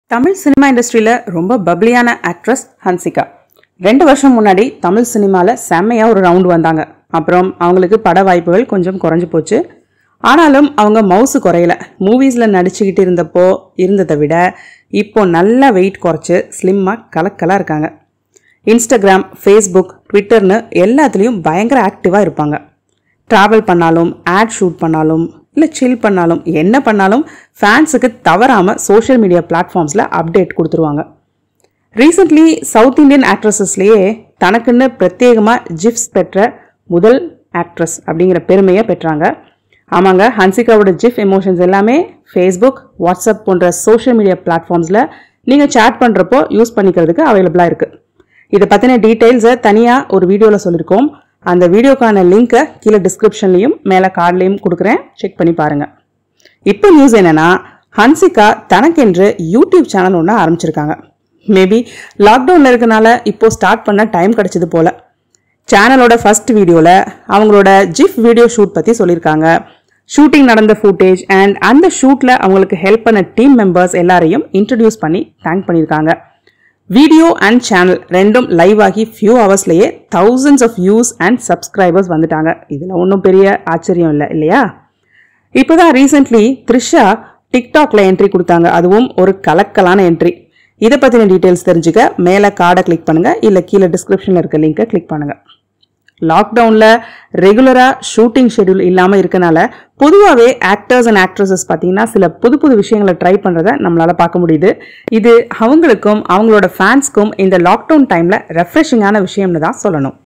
தனக்கென்று YouTube channel aarambithar Hansika Motwani
தமிழ் சினிமா இண்டஸ்ட்ரியில் ஒரு பப்லியான நடிகை ஹன்சிகா. இரண்டு வருடத்திற்கு முன்னர் தமிழ் சினிமாவில் கொடி கட்டிப் பறந்தவர் நடிகை ஹன்சிகா.

இன்ஸ்டாகிராம், பேஸ்புக், ட்விட்டர் என்று அனைத்திலும் மிக ஆக்ட்டிவாக இருக்கிறார் நடிகை ஹன்சிகா.

சமீப காலத்தில் தென் இந்திய நடிகைகளிலேயே தனக்கென்று பிரத்யேகமாக GIFs பெற்ற முதல் நடிகை என்கிற பெருமையைப் பெற்றவர் ஹன்சிகா.

இன்றைய செய்தி என்னவென்றால், ஹன்சிகா தனக்கென்று ஒரு YouTube Channel ஆரம்பித்துள்ளார். லாக் டௌனில் இருப்பதனால் சேனல் ஆரம்பிக்க நேரம் கிடைத்த்திருக்கலாம்.